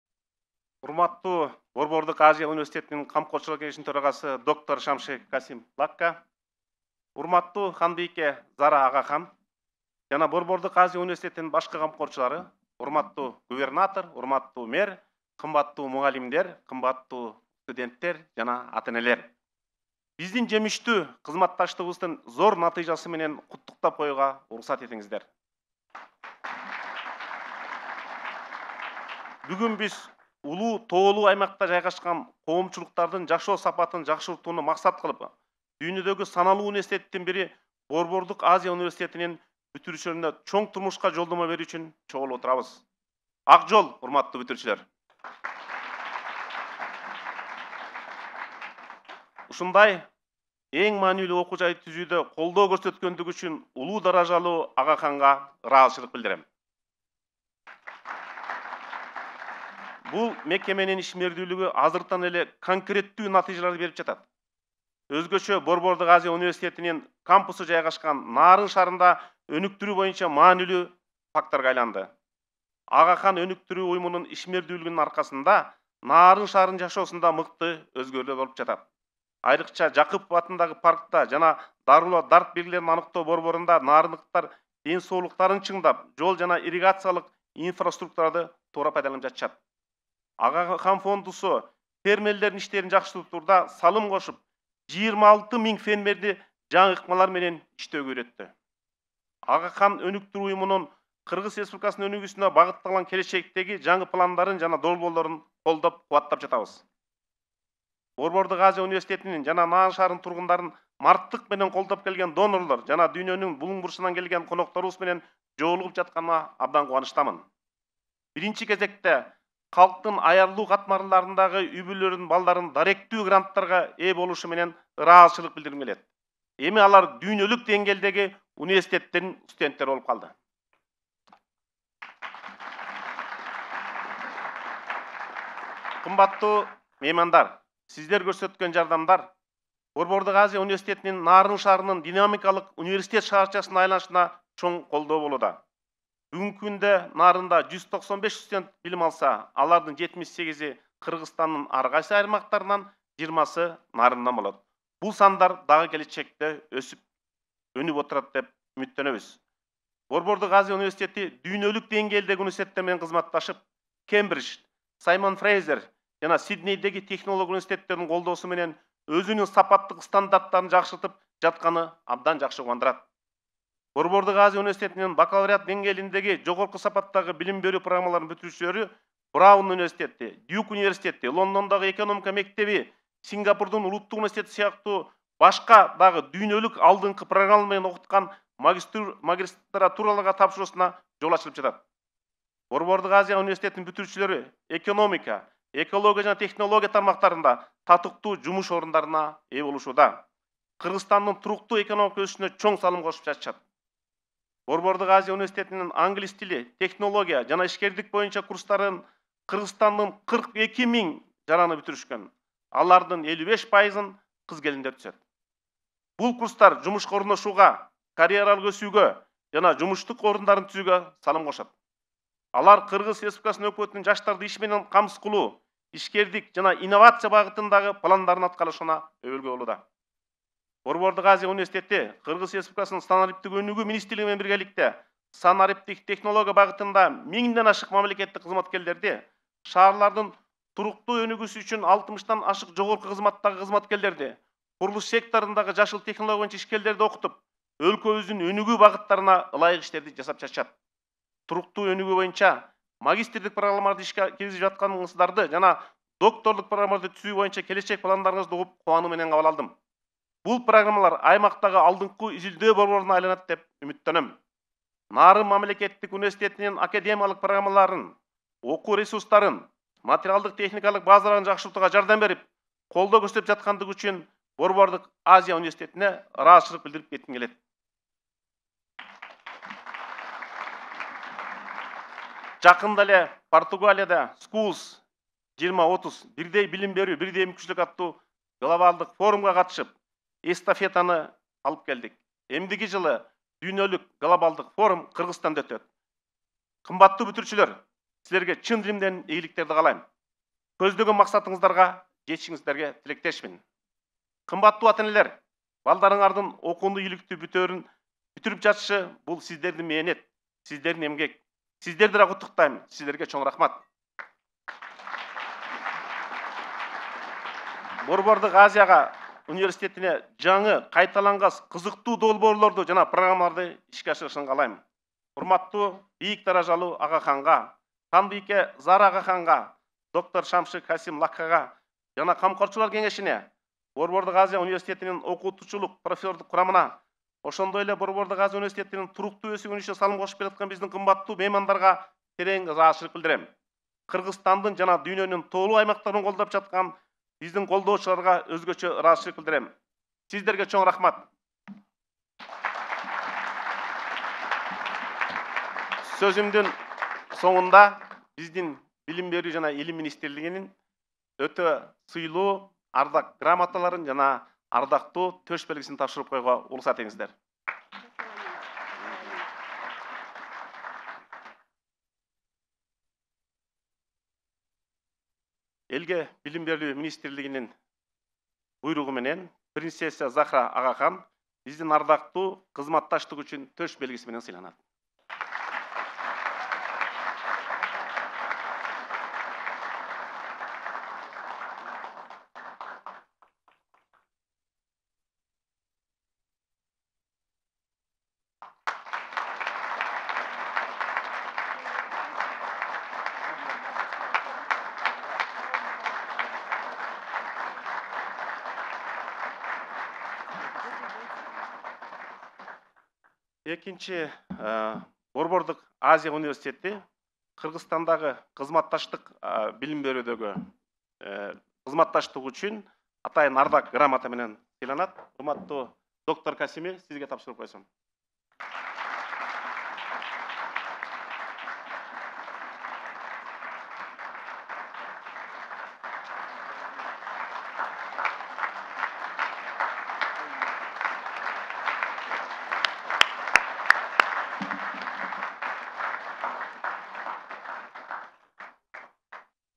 Speech by His Excellency Kanybek Imanaliev, Minister of Education and Science of the Kyrgyz Republic
Central Asia, 16 June 2023 - Amid the majestic mountains of the Pamirs and Tien Shan, the graduation ceremonies at the two University of Central Asia (UCA) campuses in Naryn, Kyrgyzstan and Khorog, Tajikistan brought together parents, faculty and distinguished guests – both in-person and via live stream – to congratulate the students for their achievements and prospects of success, share in their moment of joy and wish them well as they set forward on new paths to realise their dreams.